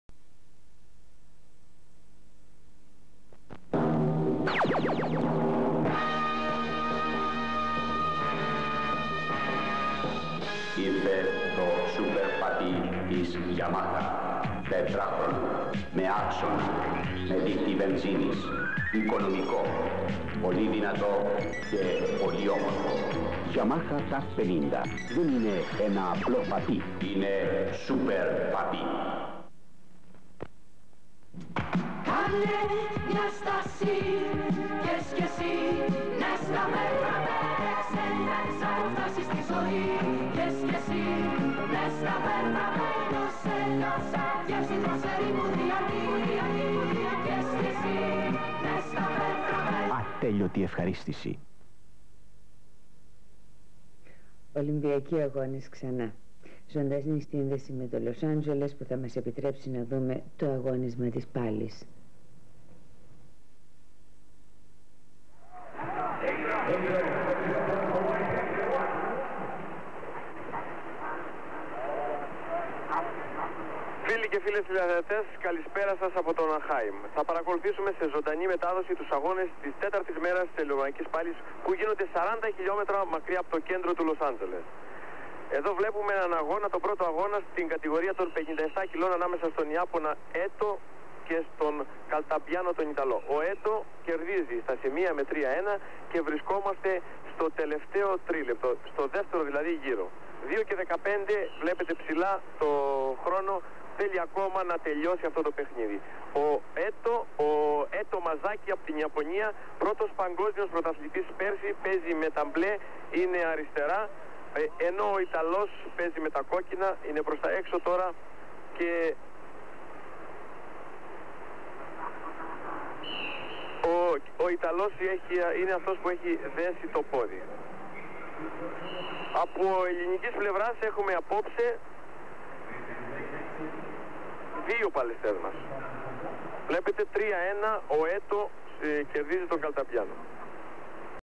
ΝΑΚΥ ΑΓΑΘΟΥ
Αναγγελία προγράμματος από την τηλεπαρουσιάστρια της ΕΡΤ Νακυ Αγαθου.
Μεταδόθηκε 01 Αυγούστου 1984.